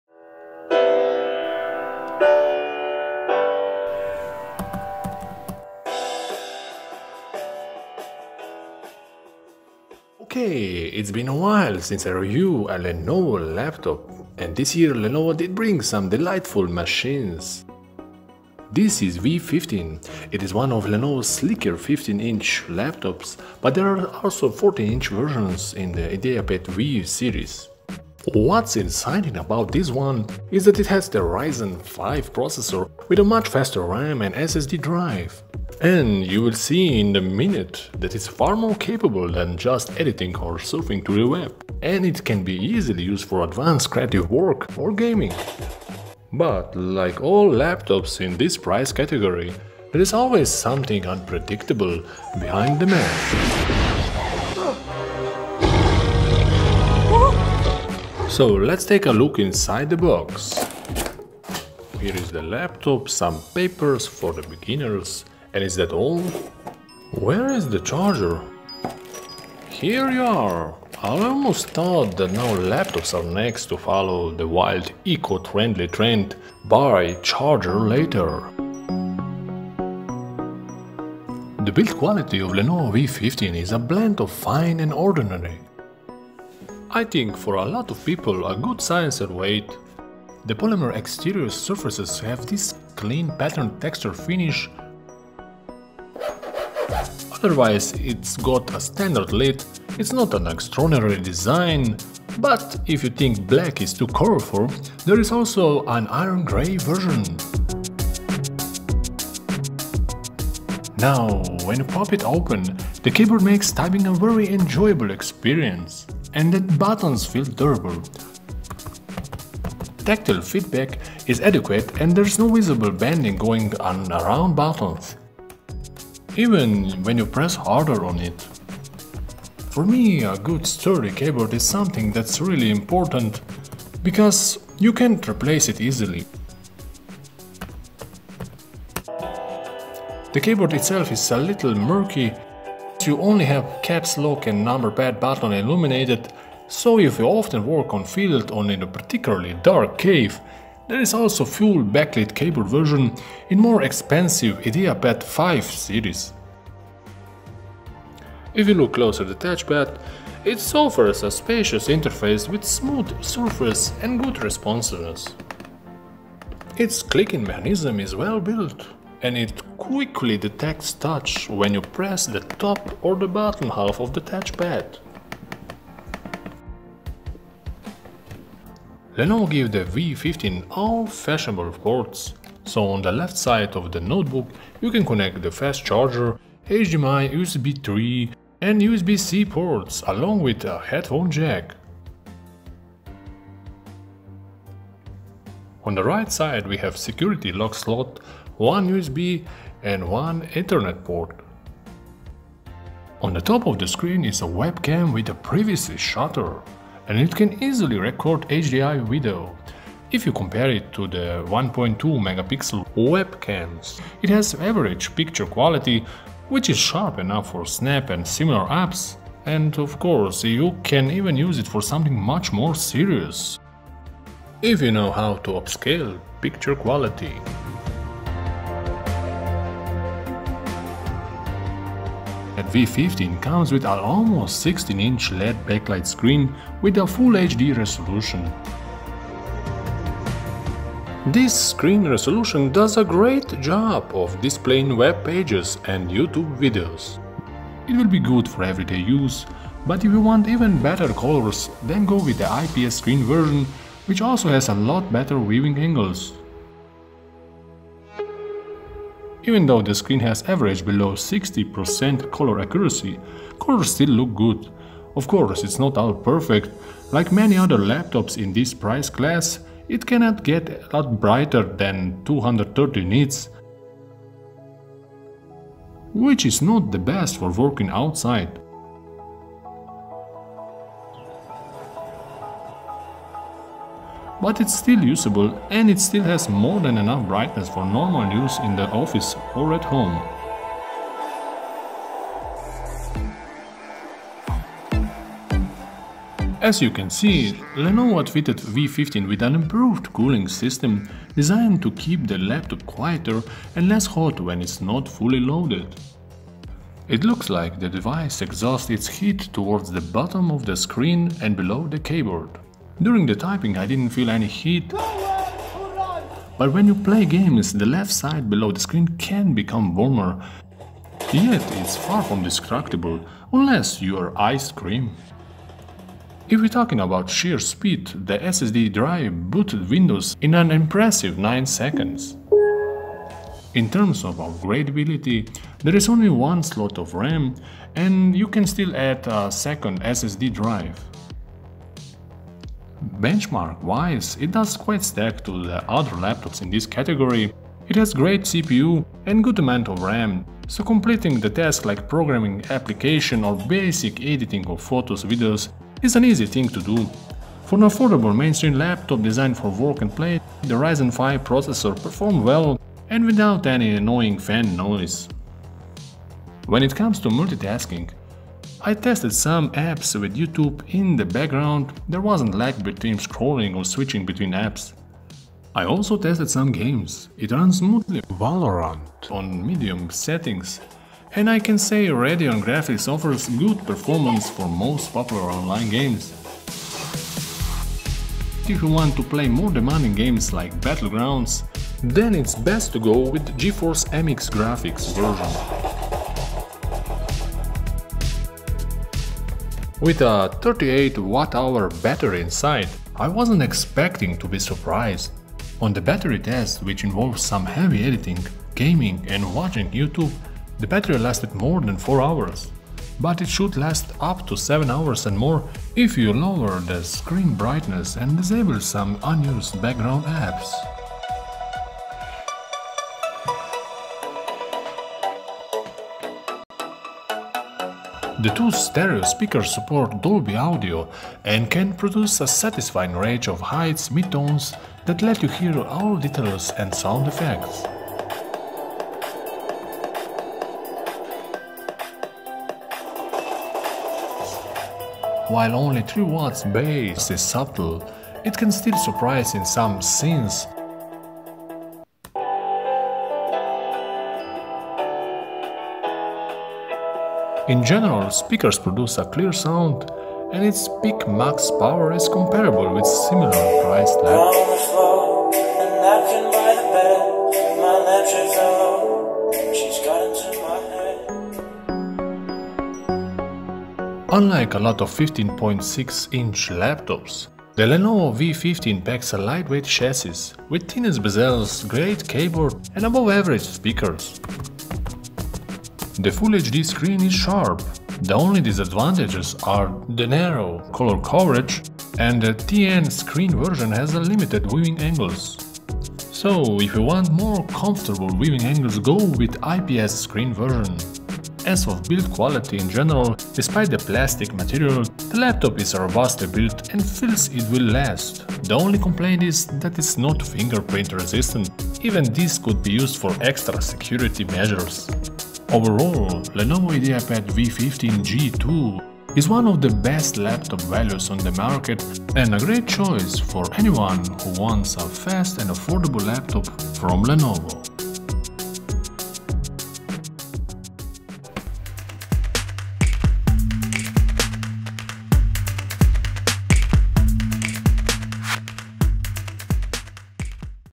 Lenovo Ideapad V15 G2 Ryzen Review
We tested the new Lenovo Ideapad V15 G2 ALC with 15.6 inch screen and latest Ryzen 5 5500U processor. It has impressive multi-core performance with 12 threads and a nice full hd display and keyboard.

It comes in Ryzen 5300u, 5500u, and 5700u processor variations.